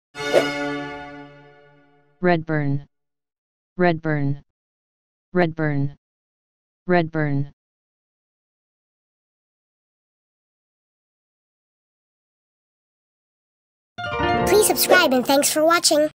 Redbourn (How to Pronounce Cities of the World)💬⭐🌍✅
This video is about "Redbourn". This video series is something special. We're covering the pronunciation of all the cities of the world.